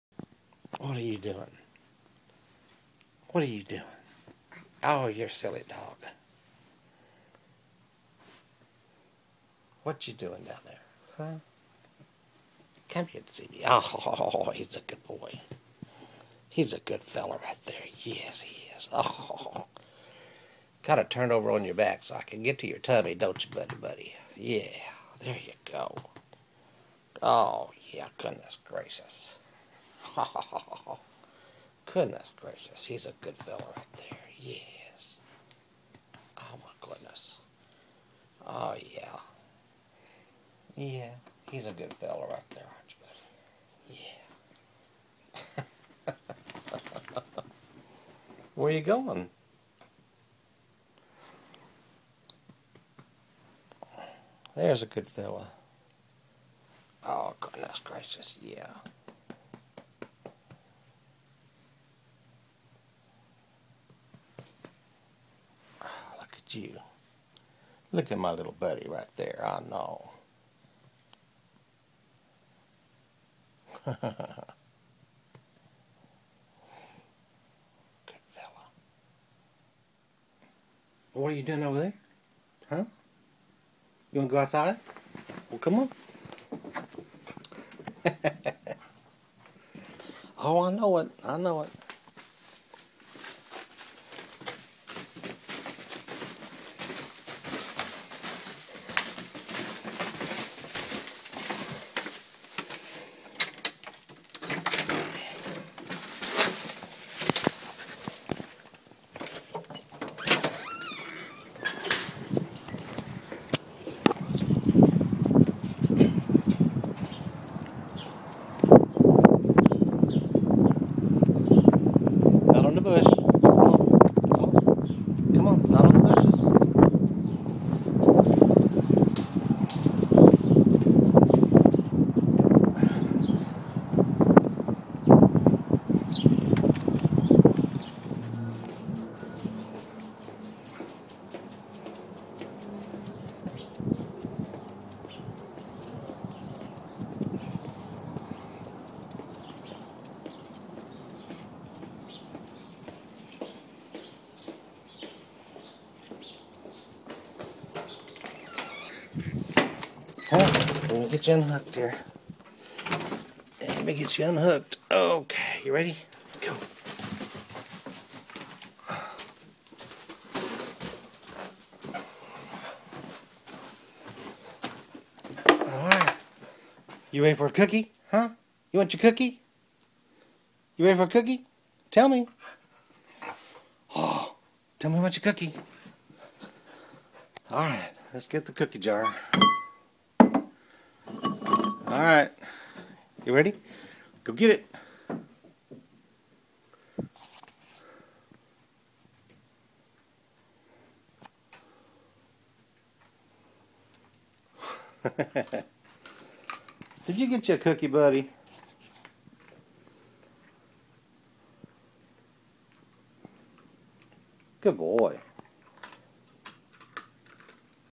Cody Wakes Up!
A typical morning when Cody stays over.